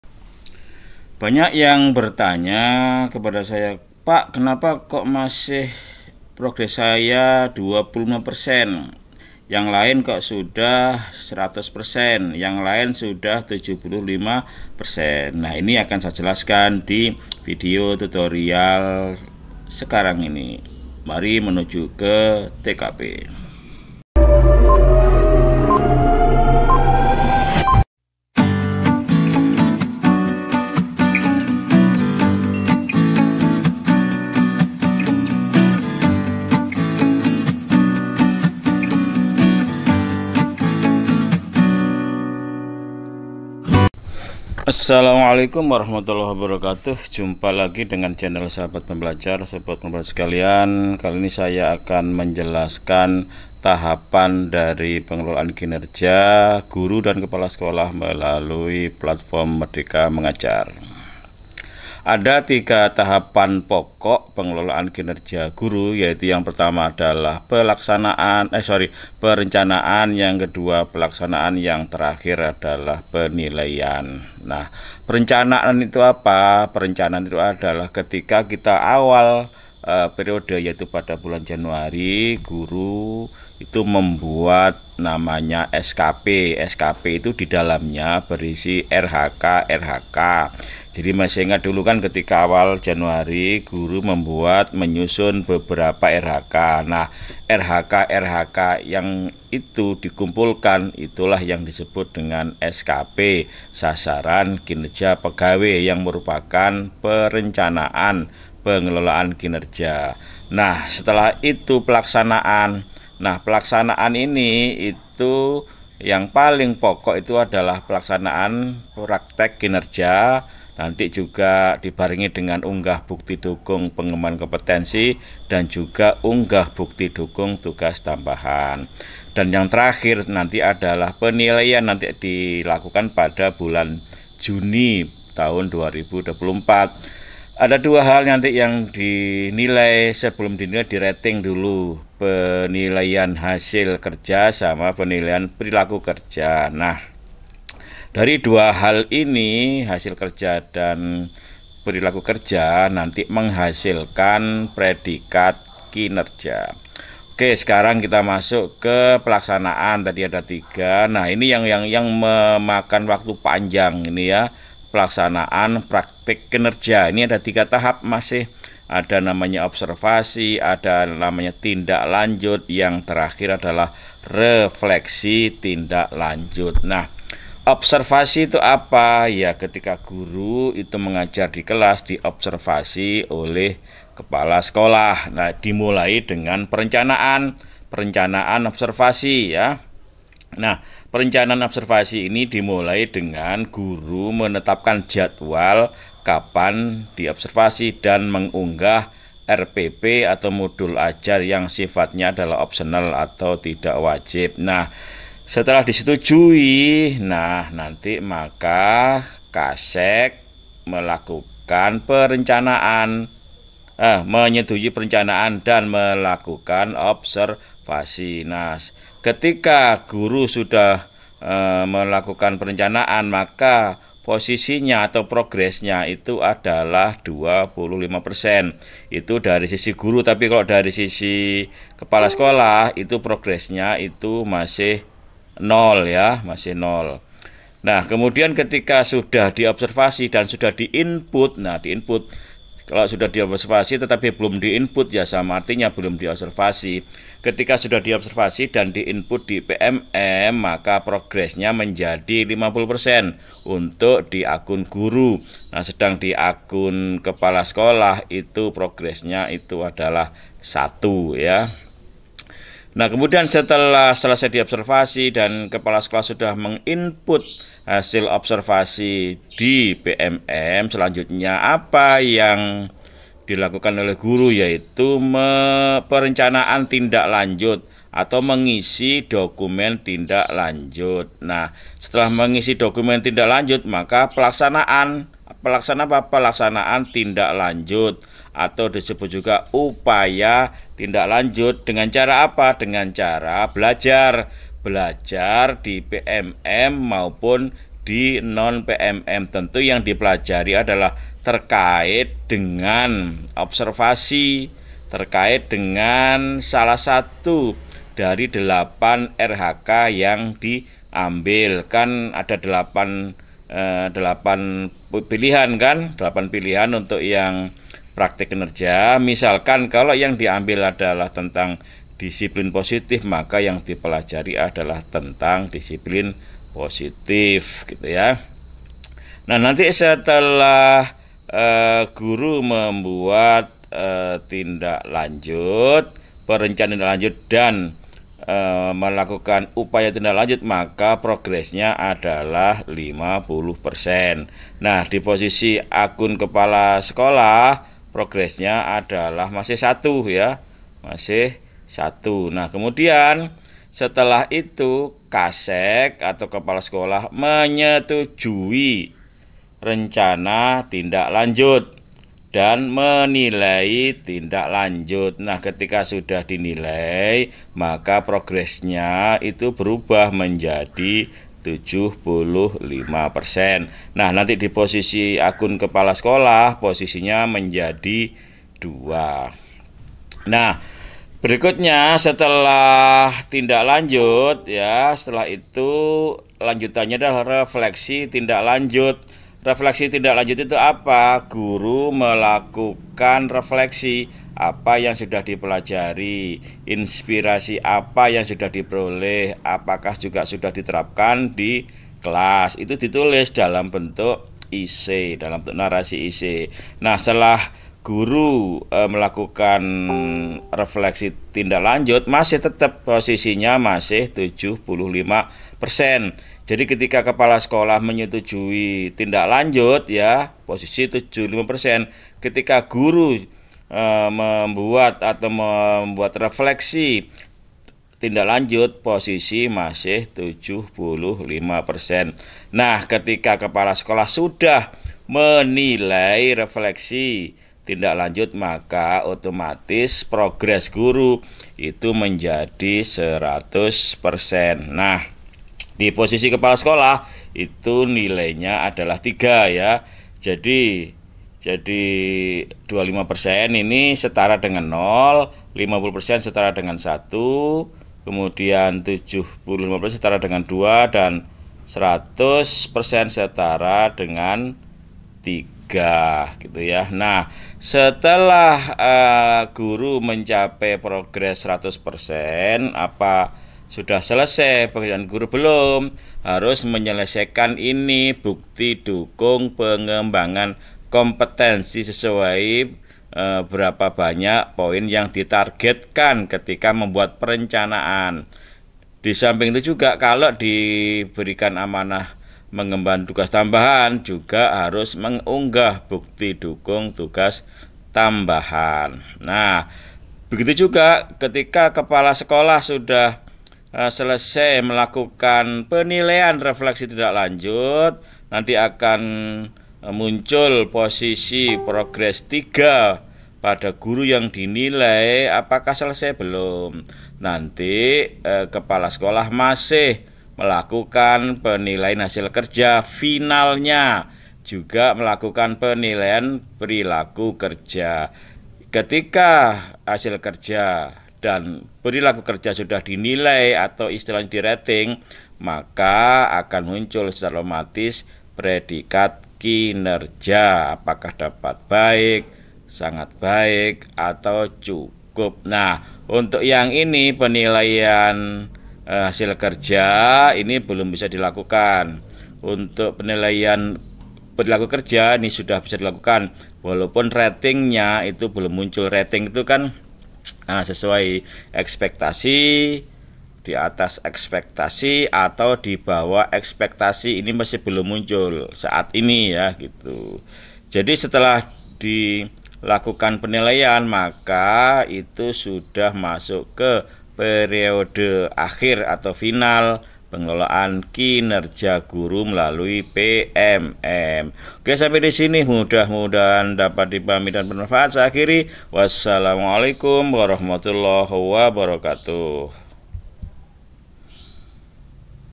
MAU 100% PENGELOLAAN KINERJA GURU VIA PMM? PAHAMI INI!
PAHAMI BAGAN PENGELOLAAN KINERJA GURU VIA PMM, AGAR TIDAK ADA YANG TERHAMBAT